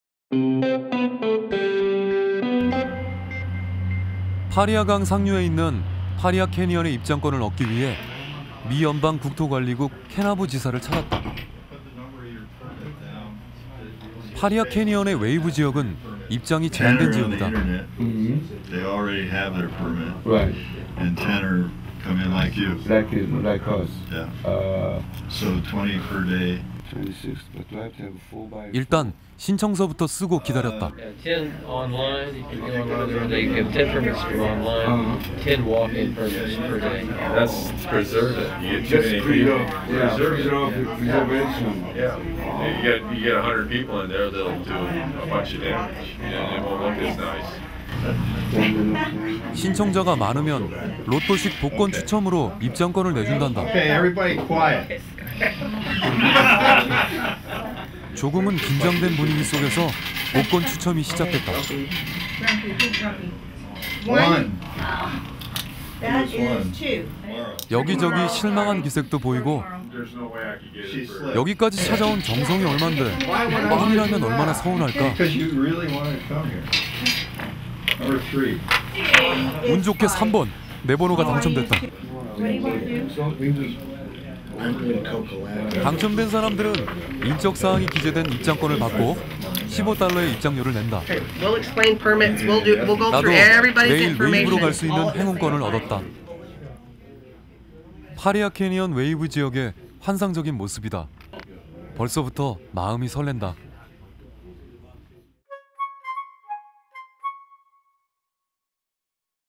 【K】USA Travel-Paria Canyon[미국 여행-파리아캐니언]입장 제한지역 웨이브/Entering the Wave/Paia Canyon/Lottery
[한국어 정보]
파이아 캐니언의 입장권을 얻기 위해 미연방 국토관리국 캐나브 지사를 찾았다. 파리아캐니언의 웨이브 지역은 입장이 제한된 지역이다. “인터넷 예약으로 10명을 정하고, 나머지 10명은 직접 방문한 신청자 중에서 추첨합니다.“ 일단 신청서부터 쓰고 기다렸다. “하루에 20명밖에 못가요. 환경을 보호하기 위해서 20명만 갈 수 있게 하는 거죠. 만약 100명 정도 사람들이 몰린다면 파손되고 오염될 겁니다. 그곳이 볼거리는 정말 많죠.” 신청자가 많으면 로또식 복권추첨으로 입장권을 내준단다. 조금은 긴장된 분위기 속에서 복권 추첨이 시작됐다. 여기저기 실망한 기색도 보이고 여기까지 찾아 온 정성이 얼만데 꽝이라면 얼마나 서운할까? 운 좋게 3번, 내 번호가 당첨됐다. 당첨 된 사람들은 인적사항이 기재된 입장권을 받고 15달러의 입장료를 낸다. 나도 내일 웨이브로 갈 수 있는 행운권을 얻었다. 파리아캐니언 웨이브지역의 환상적인 모습이다. 벌써부터 마음이 설렌다.

Federal Bureau of Land Rove found a Canadian branch of the pie to get a ticket ah Canyon. Wave Canyon area of ​​Faria is the restricted entry area. "Establish 10 kills and Internet booking, the remaining 10 patients who visited the lottery directly from the applicant." I waited to write from one application. "Is not only 20 people a day. In order to protect the environment, only 20 would you be able to go. If 100 people Moline people will be damaged and contaminated. There are LOTS of Interest truly "next thing we applicant naejun as many lottery tickets lottery style. This was a bit of a lottery started in a tense atmosphere. Locate on the hospitality here How much bang do if how sad to look here and there is also a slight disappointment? Luckily three times, my number was won. The people makes a winning entrance fee of $ 15 tickets listed under the Personal Information. I got a lucky wave to go tomorrow. Paris is a fantastic view of the wave ah Canyon area. Already seolrenda mind.

[Information]
■클립명 :아메리카017-미국15-12 입장 제한지역 웨이브에 들어가는 방법/Entering the Wave/Paia Canyon/Lottery
■여행, 촬영, 편집, 원고 : 현상윤 PD (travel, filming, editing, writing : KBS Sangyoon Hyun TV Producer)
■촬영일자 : 2011년 2월(February)

[Keywords]
아메리카,America,아메리카,미국,USA,United States of America,US,현상윤,2011,2월 February,애리조나,Arizona,Arizona